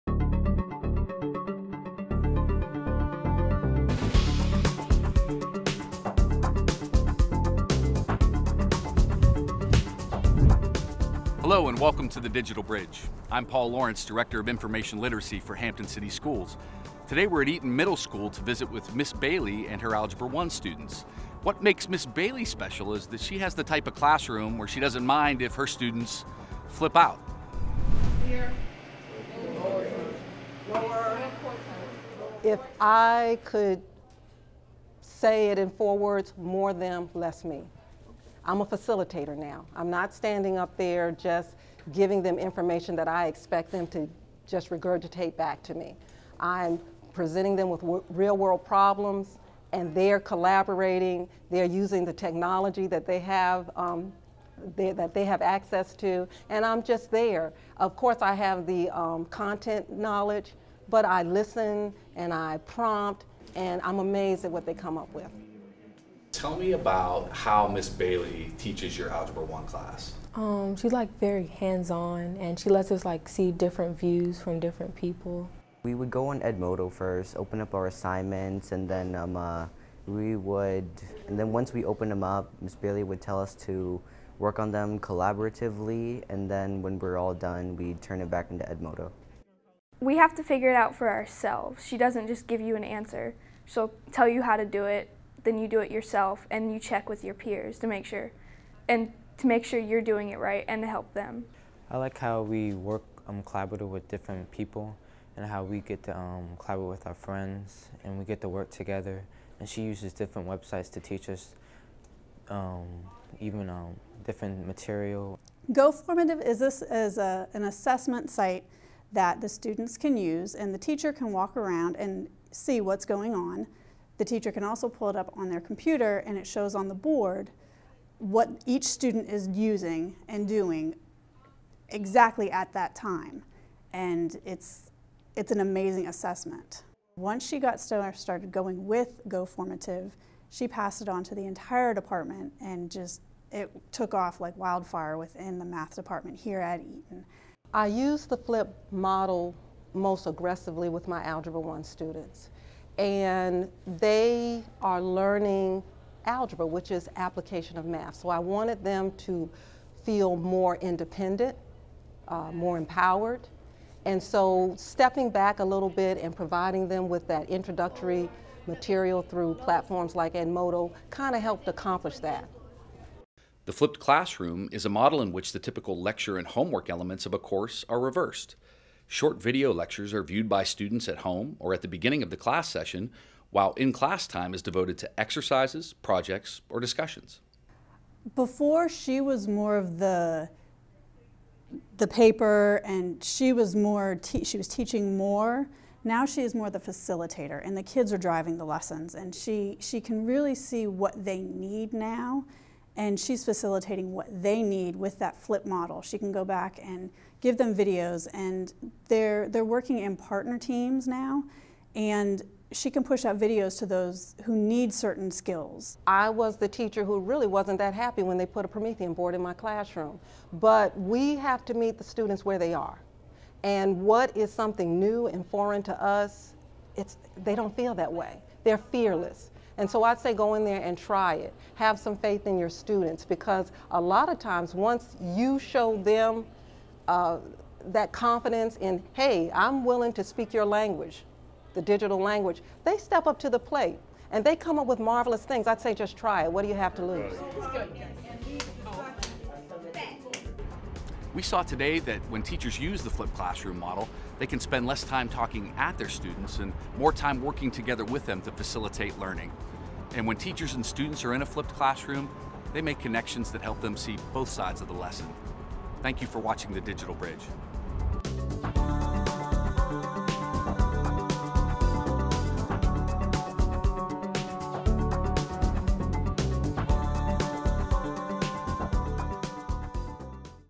Hampton City Schools - The Digital Bridge - April 22, 2016
We visit Eaton Fundamental Middle School on this edition of The Digital Bridge to see how a flipped classroom works for students and teachers.